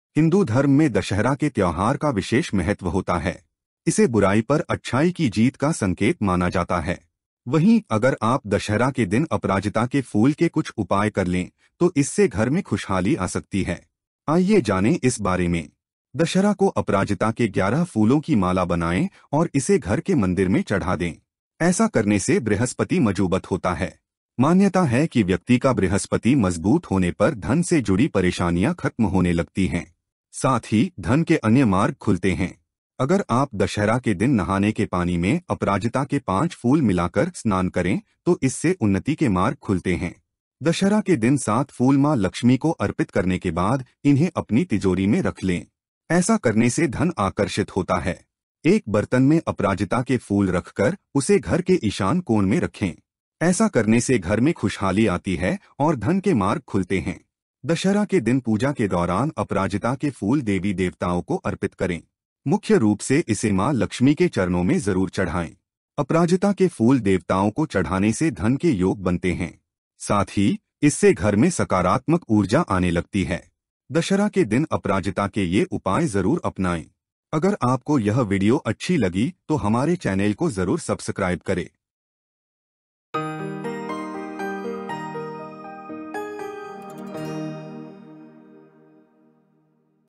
दशहरा के दिन अपराजिता के फूलों के उपाय से धन समृद्धि | Pradeep mishra ji ke upay
दशहरा के दिन अपराजिता के फूलों के उपाय से धन समृद्धि
दशहरा स्पेशल: धन के द्वार खोलें अपराजिता के फूलों के साथ

"In this special video on the occasion of Dussehra, discover the secrets of prosperity and wealth with the powerful symbolism of Aparajita flowers. Learn how to create a garland of 11 Aparajita flowers and offer it in your home temple to strengthen the influence of Jupiter, believed to alleviate financial troubles. Explore unique rituals like bathing with a blend of Aparajita flowers on Dussehra day, which is said to open the doors to progress and success. Discover the significance of dedicating seven Aparajita flowers to Goddess Lakshmi and placing them in your treasury for attracting wealth. Follow practical tips like keeping a vessel with Aparajita flowers in the northeast corner of your home to invite happiness and financial opportunities. Join us in the worship rituals, especially dedicating Aparajita flowers to the deities, and witness the positive energy it brings to your home. Embrace the auspicious practices of Dussehra with these Aparajita remedies for wealth and prosperity.